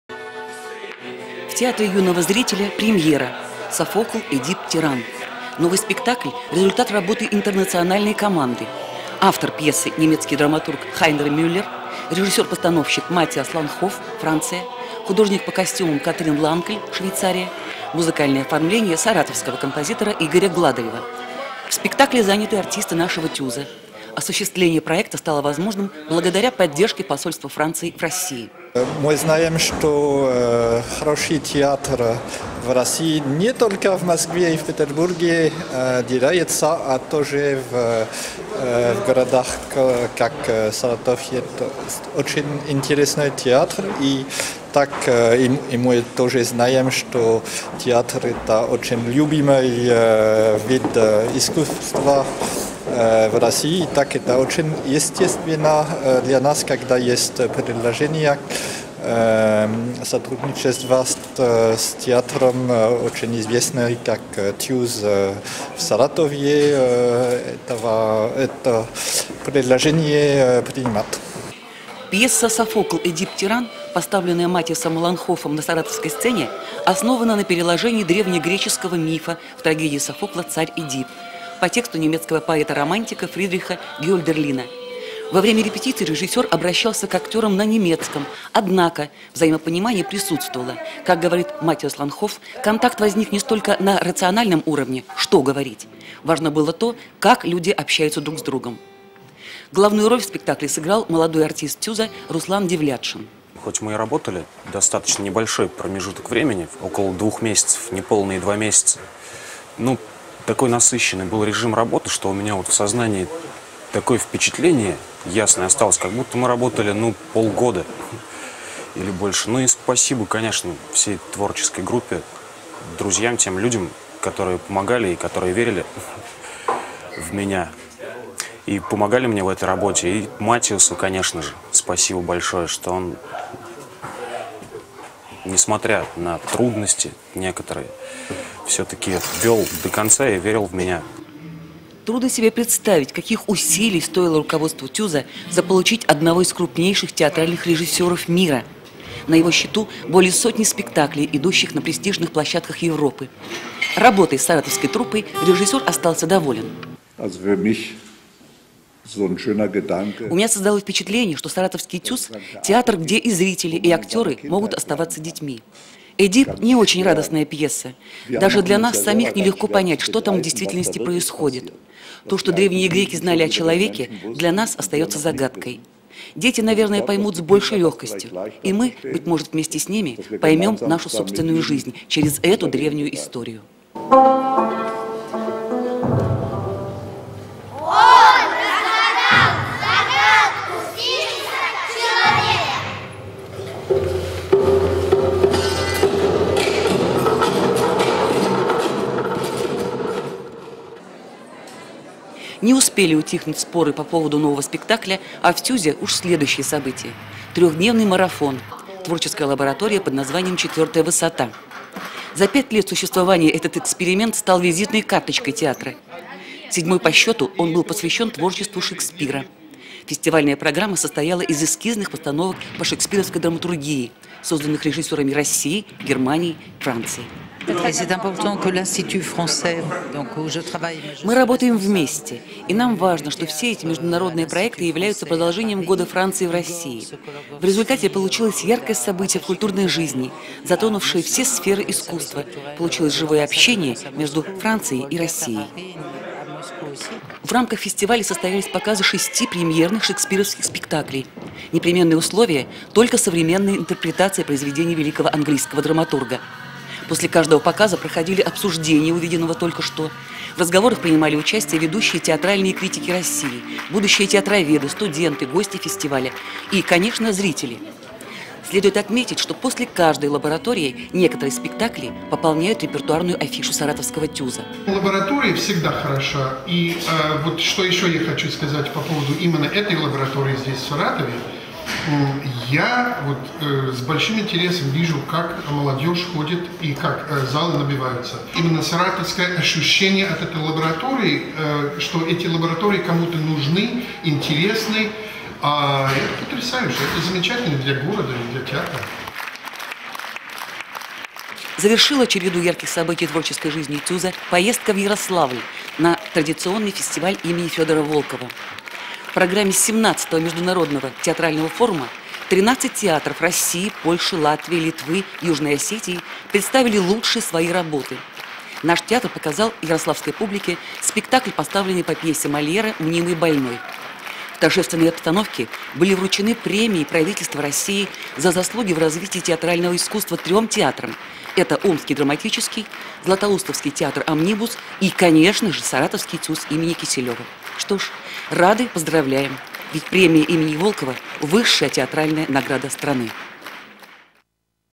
ТЮЗ в октябре, ноябре 2011г.
События в ТЮЗе Киселева в октябре, ноябре 2011г.
Постановка "Софокл. Эдип, тиран". Шекспировский фестиваль "Четвертая высота". ТЮЗ на Волковском фестивале в Ярославле со спектаклем "Мнимый больной".